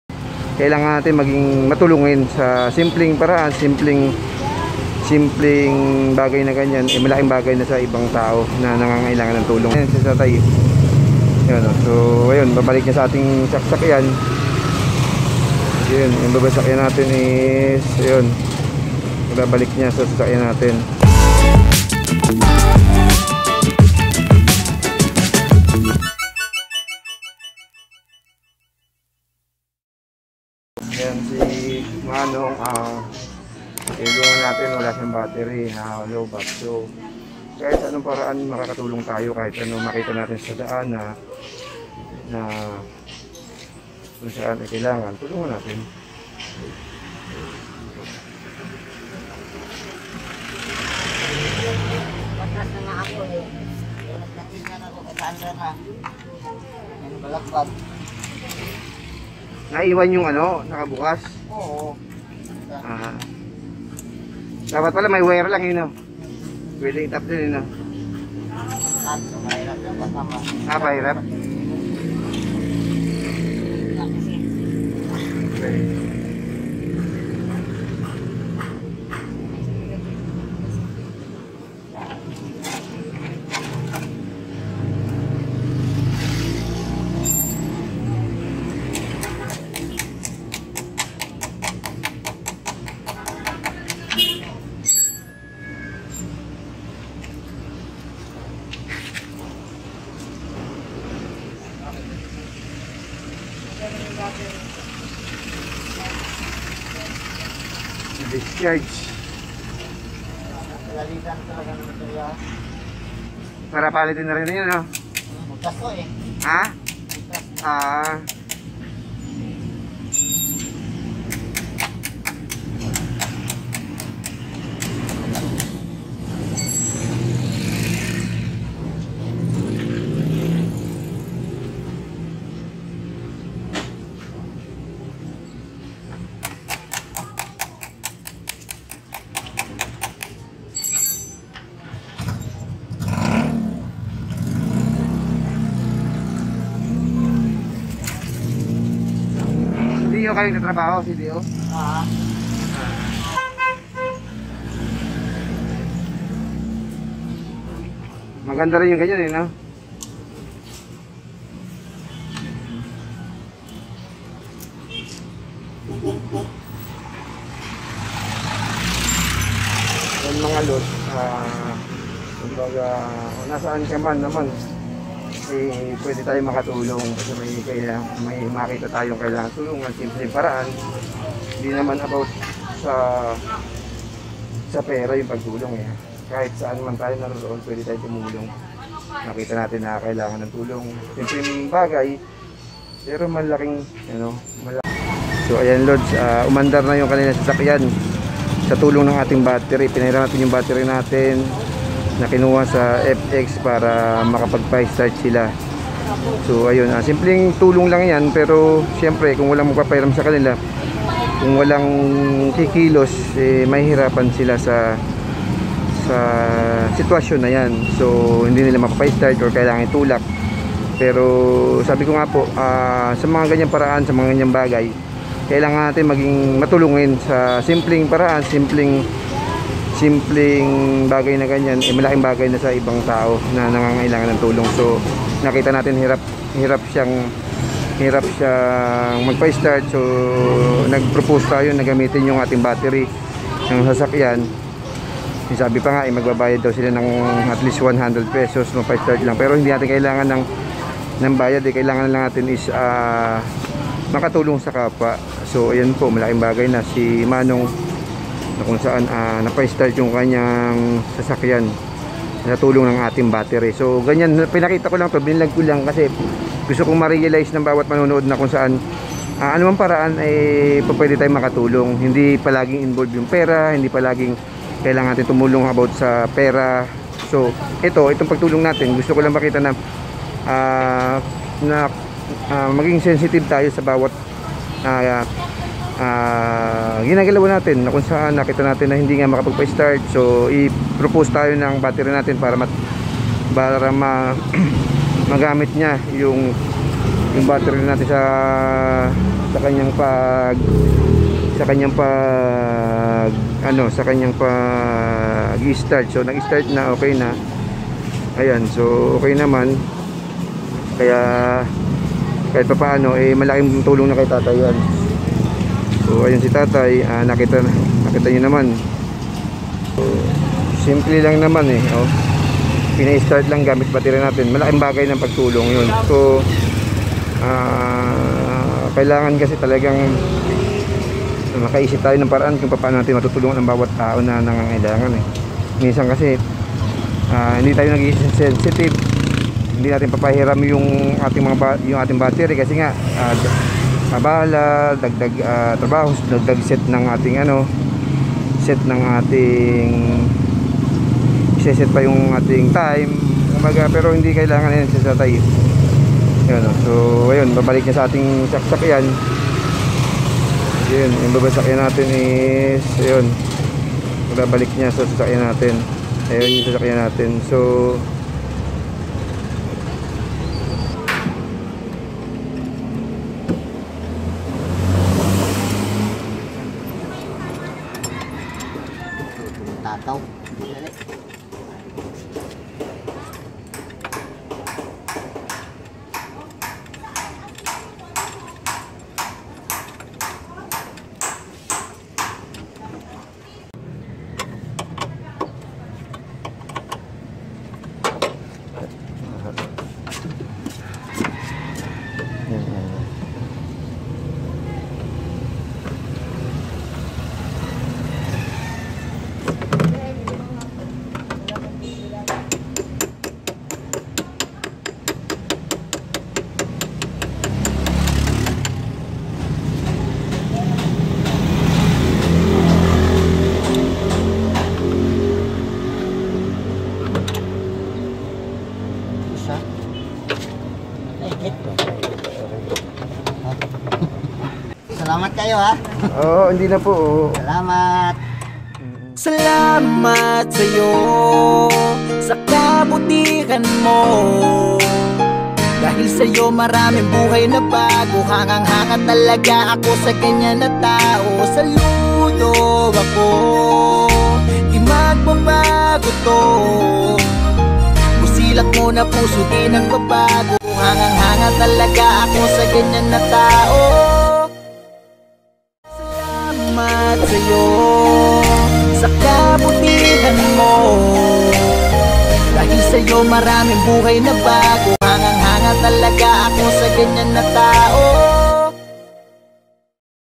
Simpleng tulong kay Manong Driver
Sa bawat nakikita natin na nangangailangan ng tulong ay wag po tayong mag dalawang isip na tulungan sila... ang bawat pagtulong natin ay kaluguran sa Panginoon...